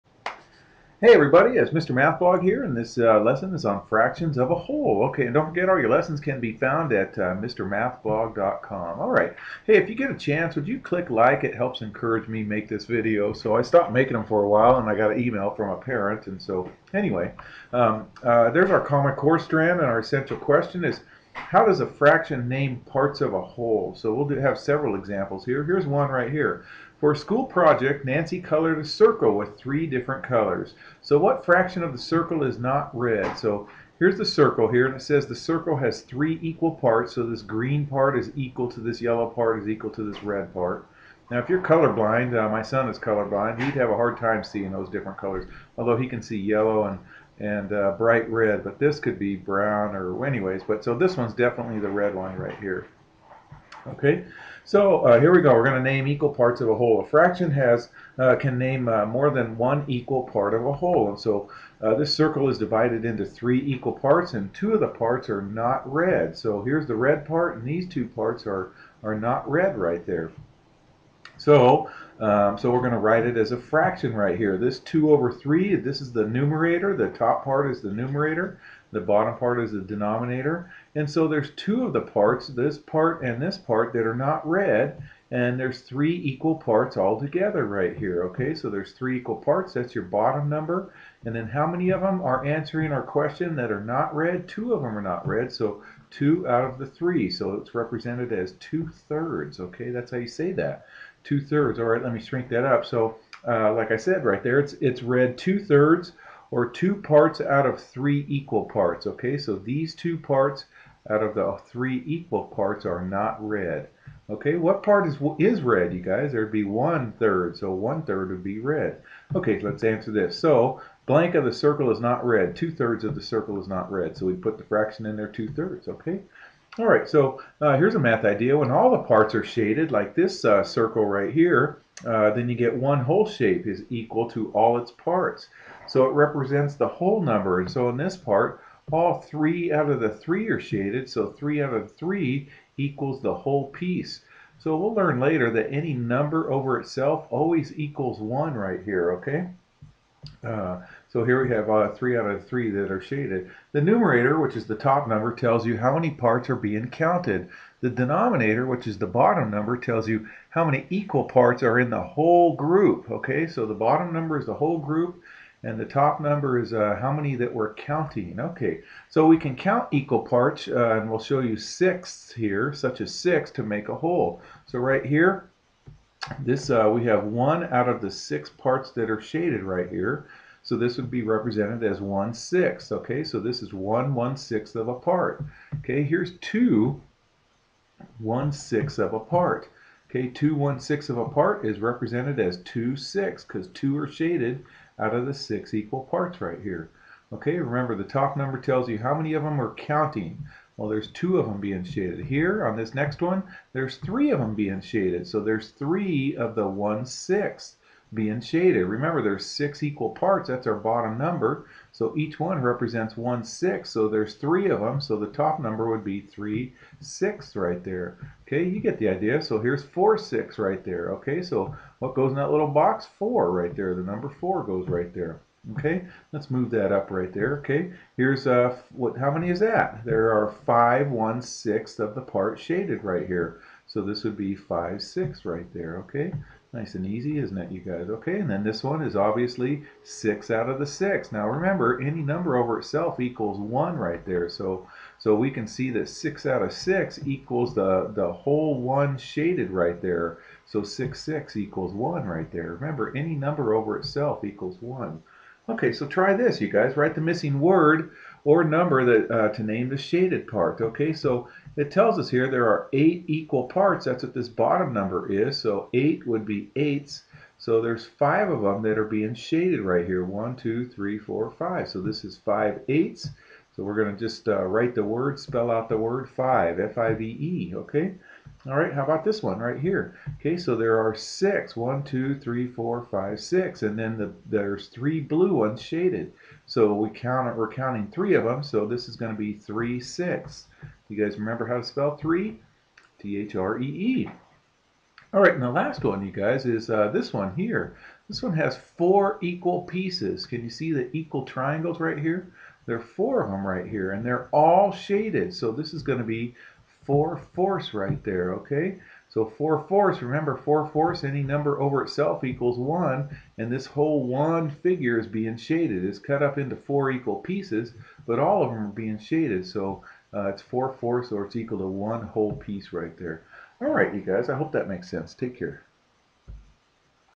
Fractions of a Whole - Lesson 8.4
This lesson shows you how to write fractions that are equal parts of a whole.